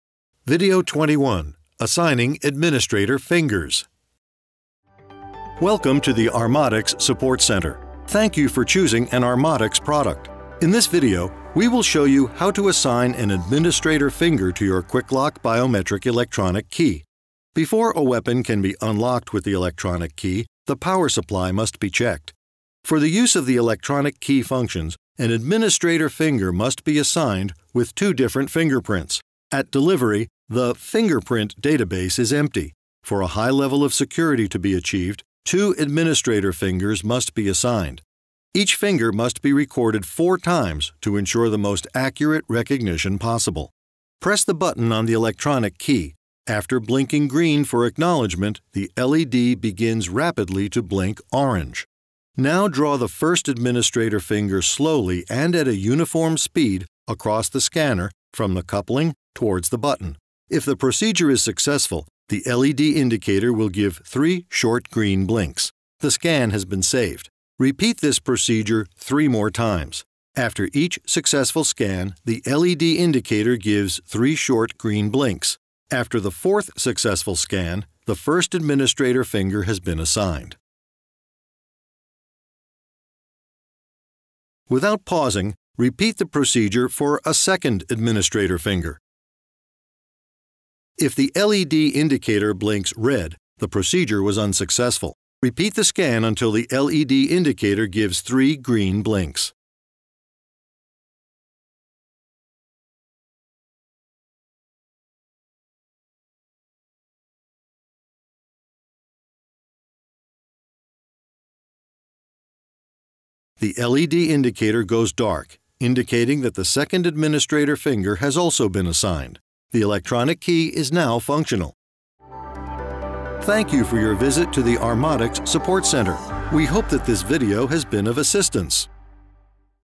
Assigning administrator finger - Armatix Quicklock system
BIOMETRICS AND ELECTRONIC KEY
The electronic key is the key to the locking element. Authentication is via PIN code or biometric fingerprint technology. A wireless key can be used to lock a large number of locking elements and weapons as required.